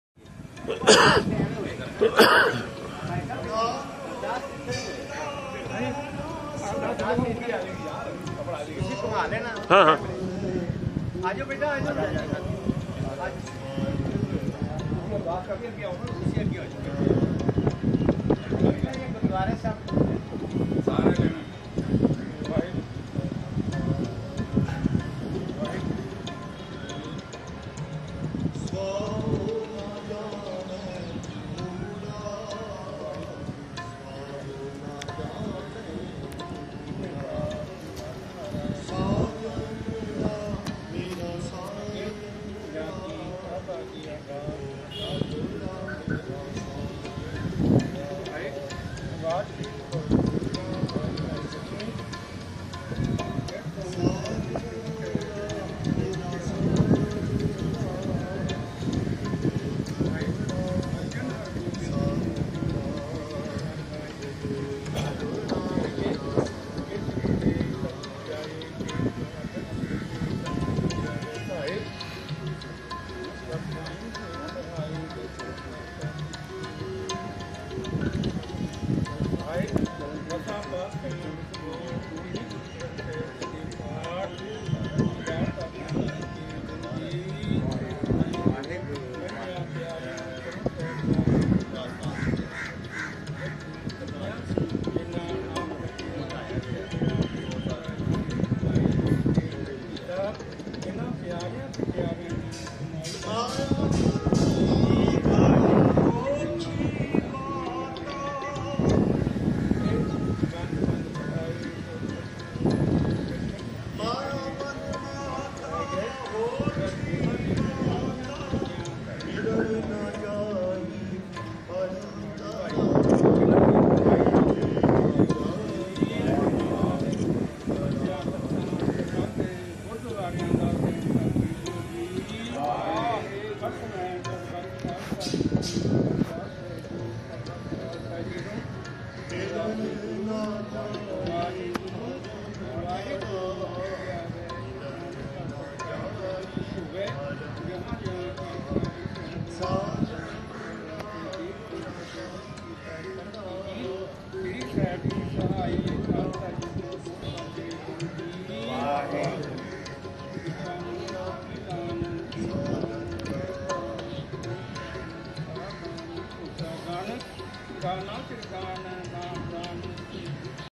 Gurcharan Singh Babbar gathered with the Sikhs at Gurudwara Shri Rakab Ganj Sahib Part- 10
श्री गुरुद्वारा साहिब को बुलडोजरों के साथ मलियामेट करने के विरोध में गुरुद्वारा श्री रकाब गंज साहिब सिखों ने इकट्ठे होकर, केंद्र सरकार और योगी सरकार के खिलाफ जमकर प्रदर्शन किया। दिल्ली पुलिस ने धारा 144 लगाकर सिखों को बाहर सड़कों पर प्रदर्शन करने नहीं दिया। सिर्फ दो सिख लीडर South block प्रधानमंत्री के दफ्तर MEMORANDUM देने गए।